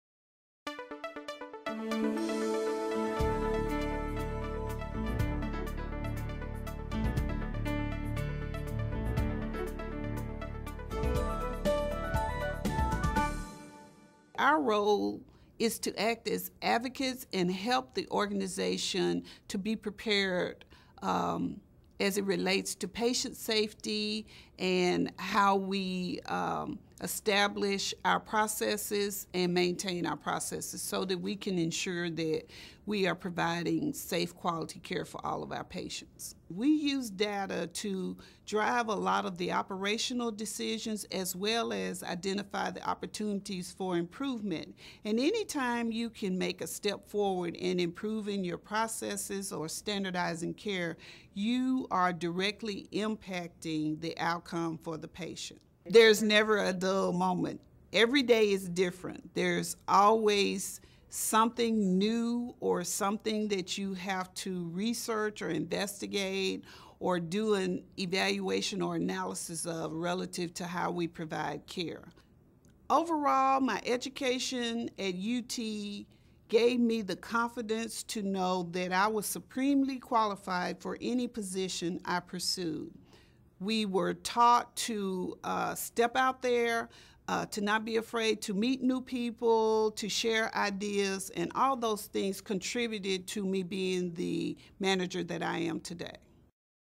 One Degree, Many Choices: Careers in HIM
The Department of Health Informatics and Information Management offers undergraduate and graduate degrees in Health Informatics and Information Management as well as a post-baccalaureate certificate in Health Informatics and Information Management. Health Information is one of the fastest growing professions in the country based on the U.S. Bureau of Labor Statistics. According to the American Health Information Management Association, there are more than 120 different job titles that health information professionals hold demonstrating the wide range of skills and opportunities for the health information professional.